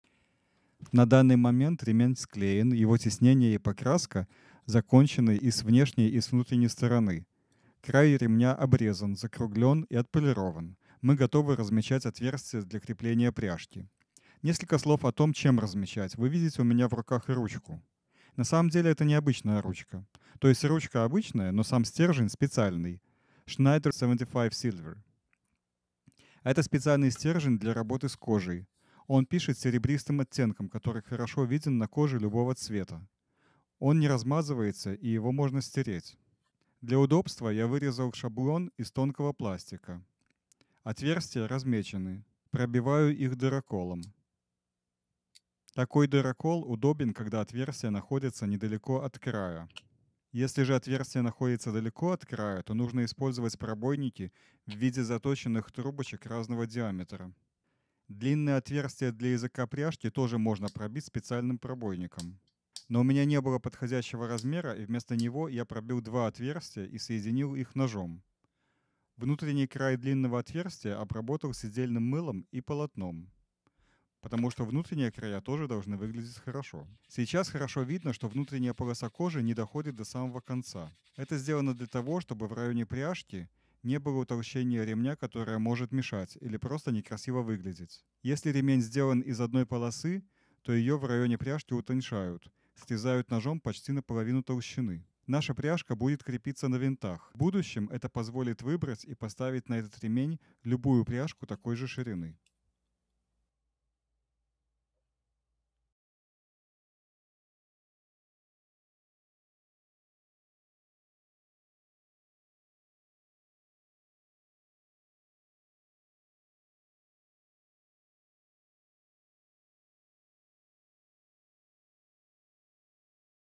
Making a custom leather belt #5 - как сделать кожаный ремень - buckle fasteners
This is the #5 of 10 videos that explain the process of making a custom made carved belt.

There are plenty of similar tutorials for English-speaking audience, so this one is made to help Russian speakers.

English subtitles are included - enable them by clicking on the (CC) button under the video or see the transcript below.

Это пятый из десяти видео файлов демонстрирующих процесс создания кожаного ремня.

Transcript:
Currently belt glued. The stamping and painting are done on both front and back sides.

Edges are cut off, rounded and polished.

We are ready to mark the holes for fastening the buckle.

A few words about the marker. You see a pen. But actually this is not an usual pen .

The pen itself is usual, but the insert is Schneider 75 Silver.

This is a special pen to write on leather.

Its silver tint is clearly visible on a leather of any color, it does not smudge and it can be erased.

For convenience I made a thin plastic pattern.

Holes were marked, then punched.

A  rotary punch is convenient when the holes located close to the edge .

If the hole is far from the edge, you need to use drive punches.

The buckle hole can be made with an oblong punch.

But I did't have the correct size, so instead I punched two holes and connected them with a knife.

The inner edge was polished with the saddle soap and a canvas.

The inner edges should also look good.

You can see that the inner strip of leather doesn't go to the end.

This is done to prevent the belt thickening that may be inconvenient or just look ugly.

If the belt is made of a single strip of leather, it would be skived near the buckle to reduce its thickness in a half.

Our buckle is fastened with screws .

In the future, these screws will allow selecting any belt buckle of the same width.